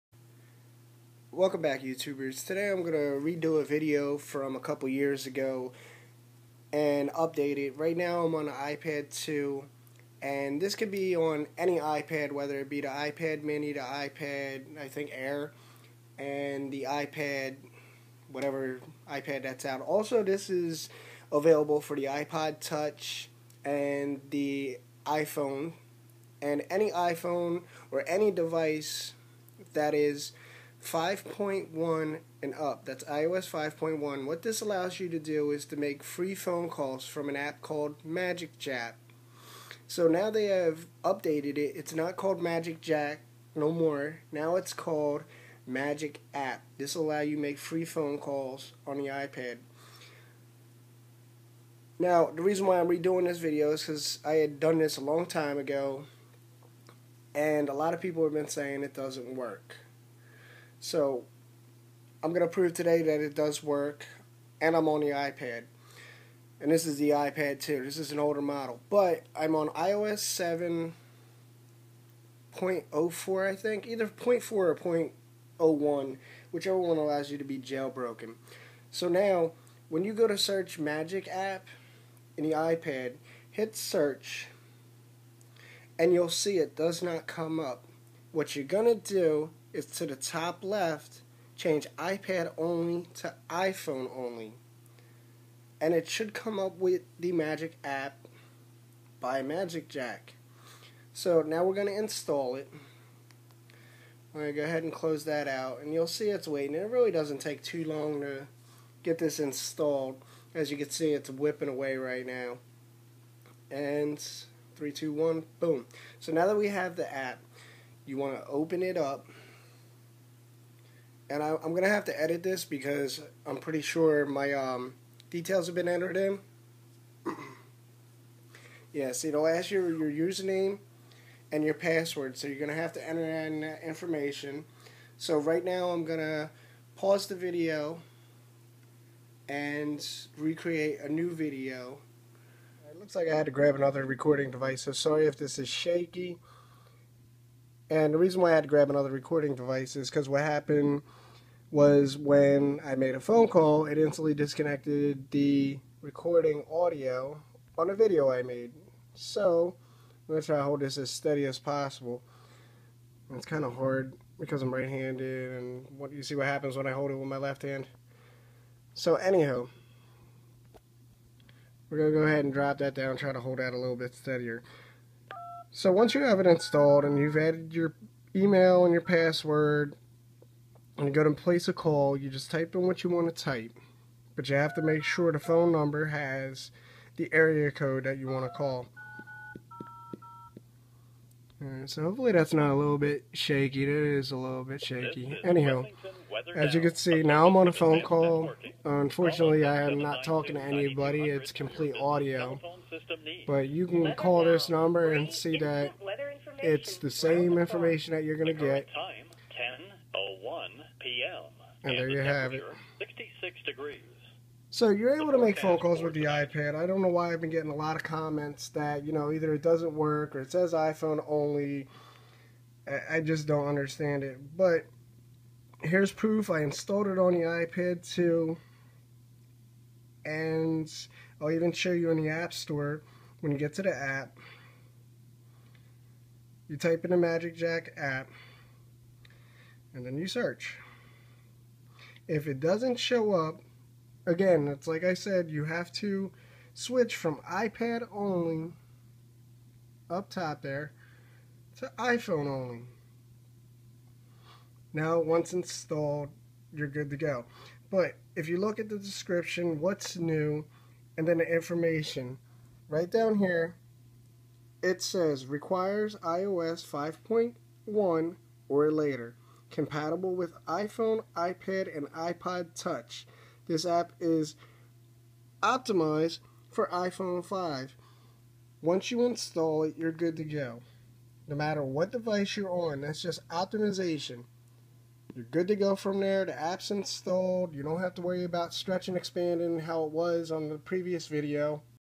Free Calls On iOS iPad iPhone an iPod touch Update Magicjack MagicApp
i have been receiving a lot of messages that this app no longer works. in this video i will show you.
how to find it.
install it.
how to log in.
how to make a free call.
this works 100% Free

installed on iPad 2

iOS 7.0.4

watch my previous 2012 free call magicjack app video for more details on this app